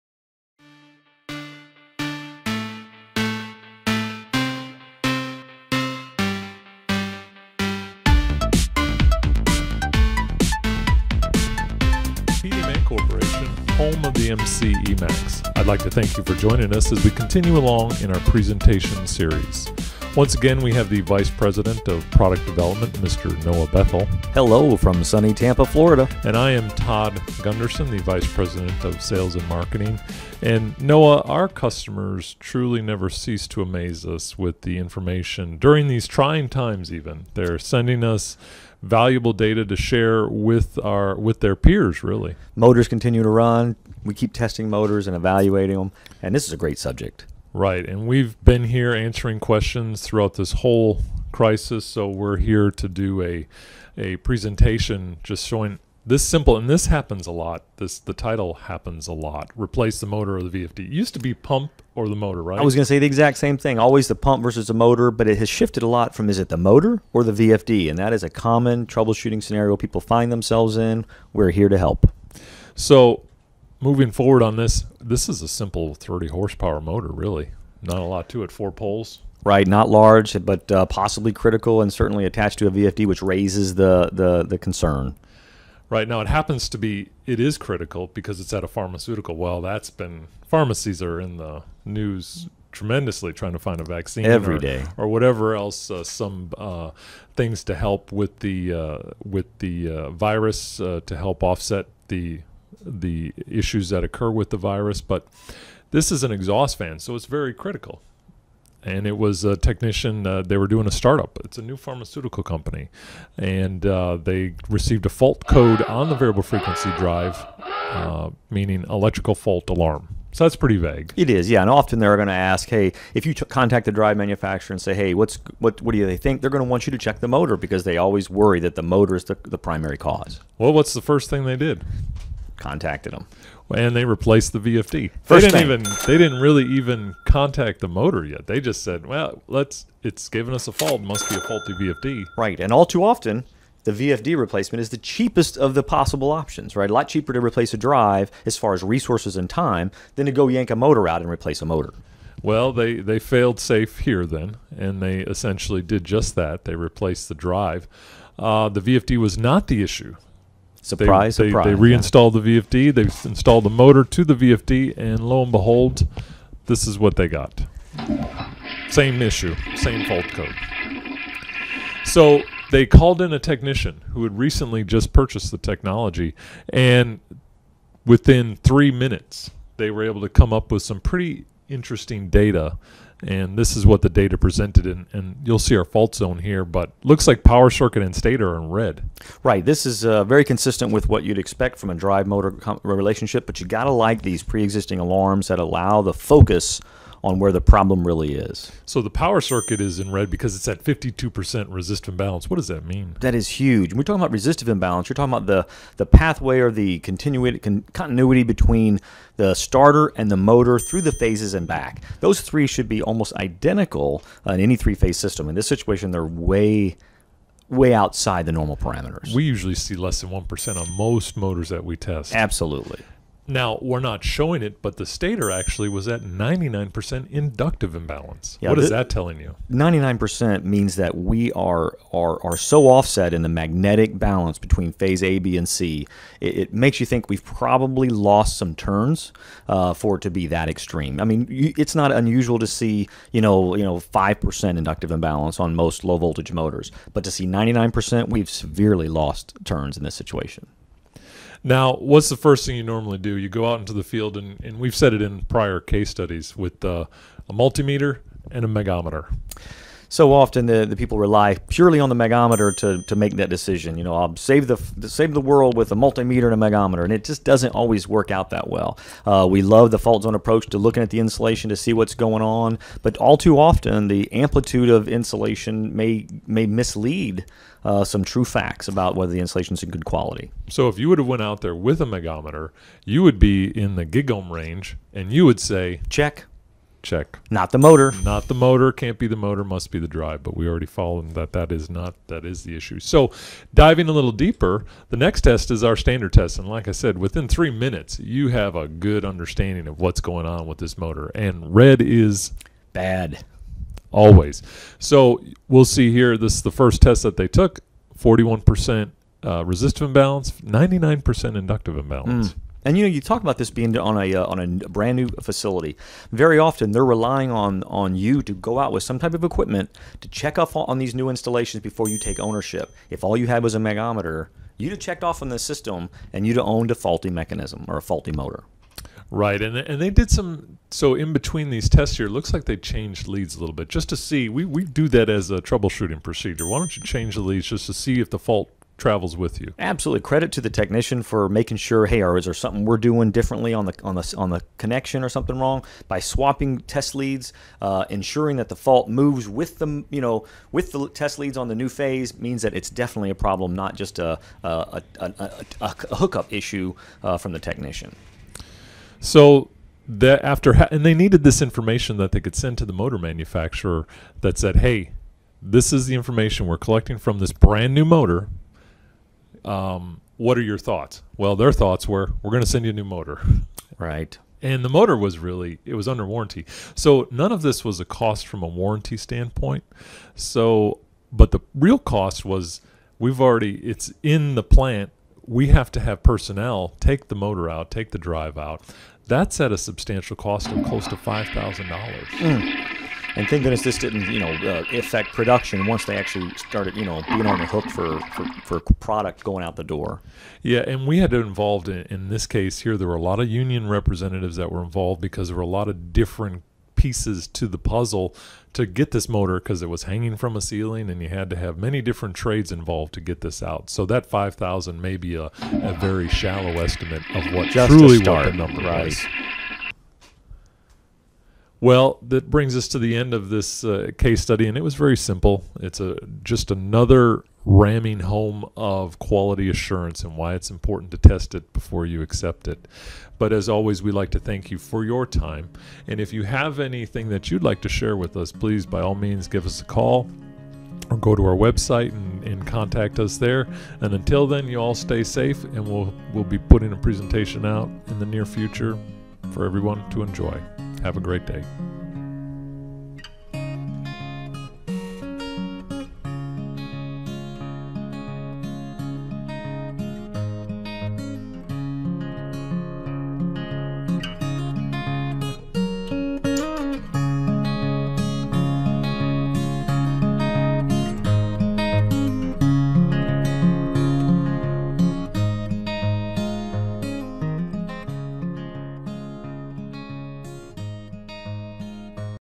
Replace The Motor or the Variable Frequency Drive
Please join Noah Bethel and Todd Gunderson as they analyze data to determine if the induction motor is faulty or the VFD (variable frequency drive) is faulty